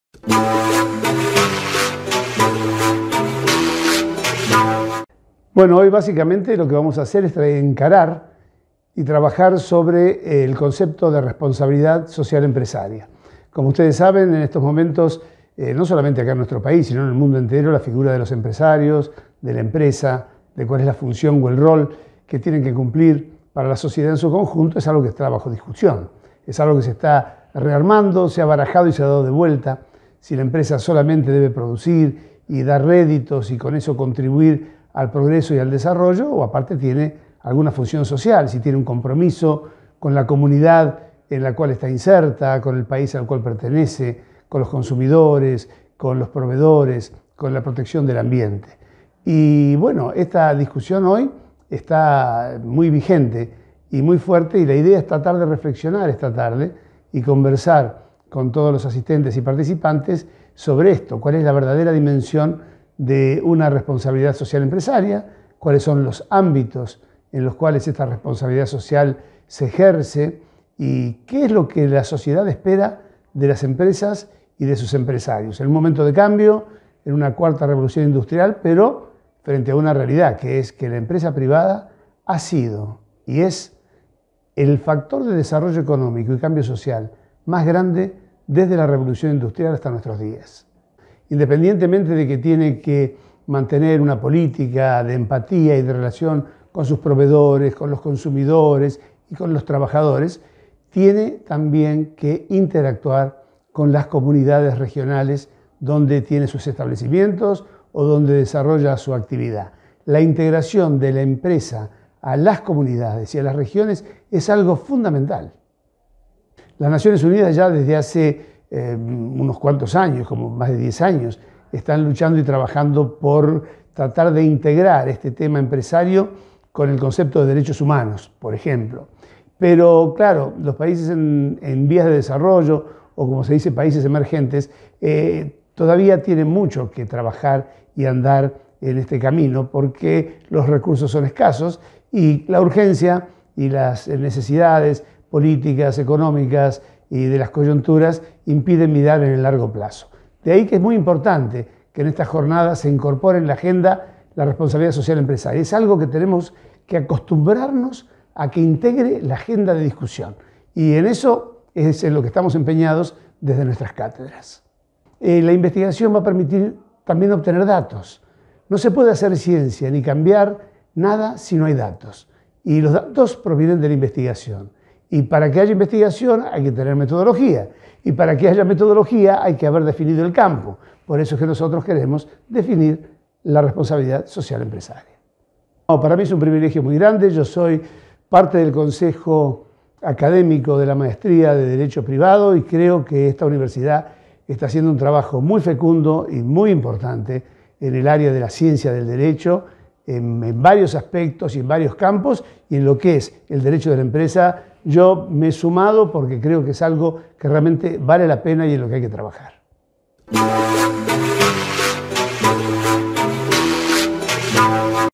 JICES: Concepto de responsabilidad social empresarial
En el marco de las Jornadas de Investigación en Ciencias Económicas, Jurídicas y Sociales (JICES), organizadas por la Facultad de Ciencias Económicas, Jurídicas y Sociales de la Universidad Nacional de Salta, habla el Dr. Daniel Roque Vitolo de la UBA sobre el concepto de responsabilidad social empresarial.